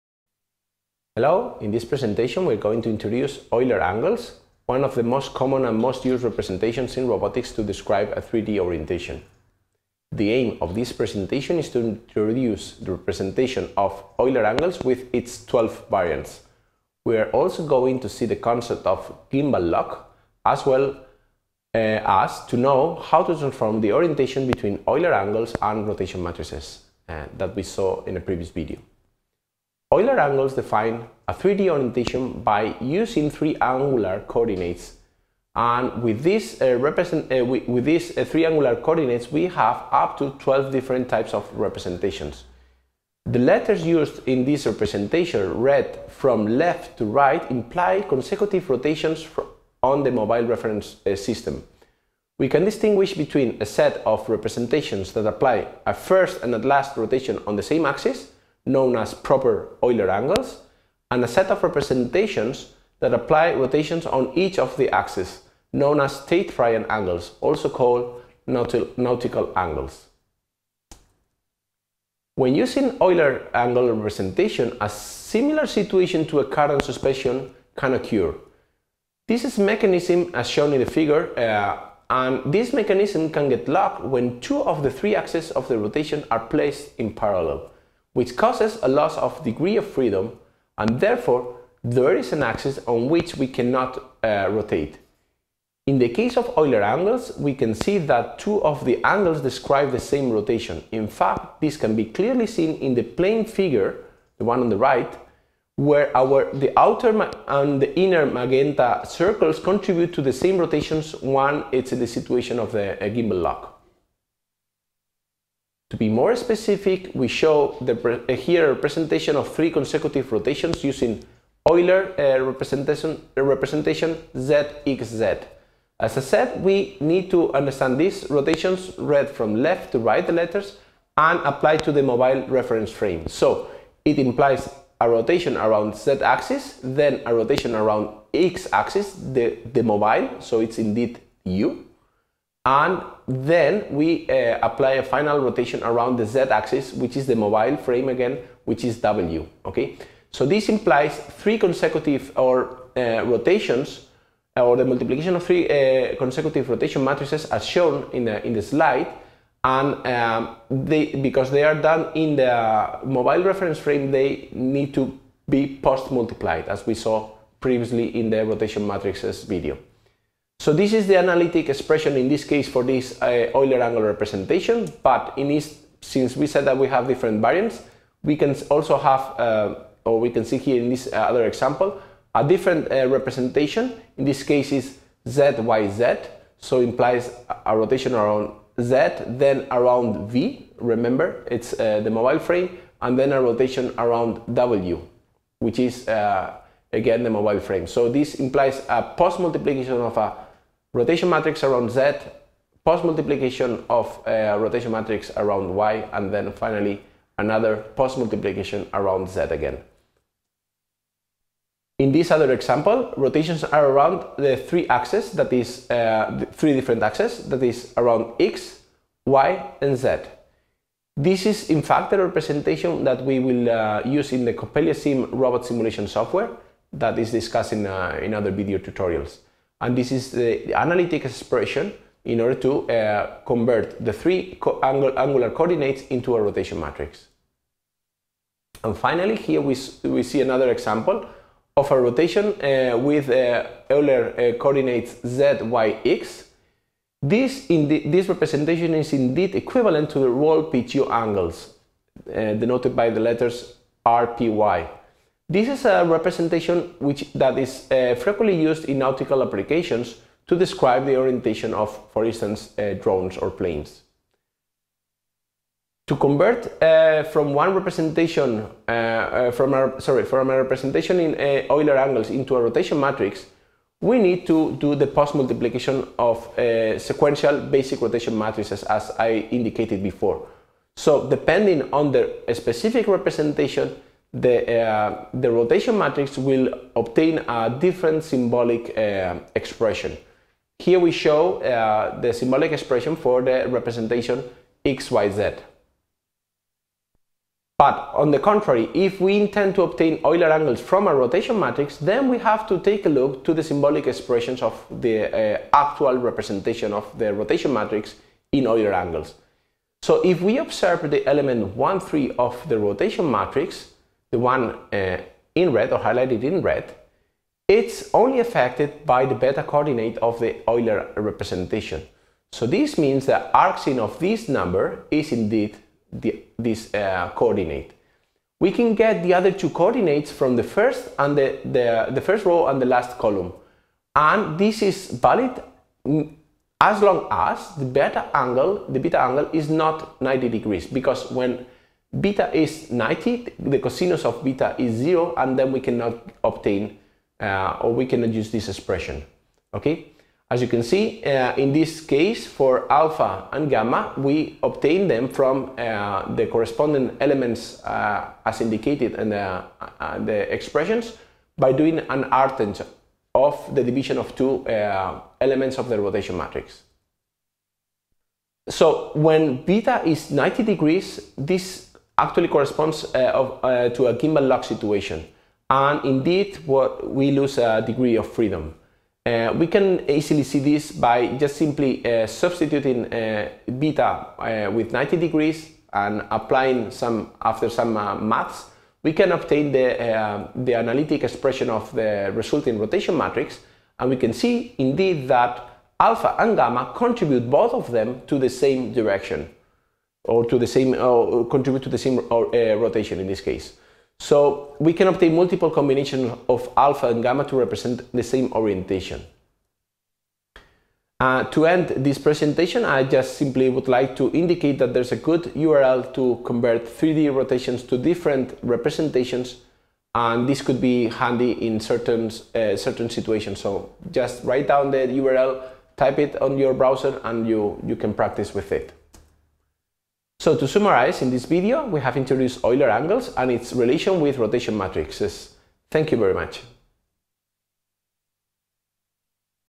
Euler Angles | Robotic Systems
This video introduces Euler angles, one of the most commonly used representation to describe a orientation in 3D.

This video is part of a set of video tutorials on robotic systems for undergraduate students.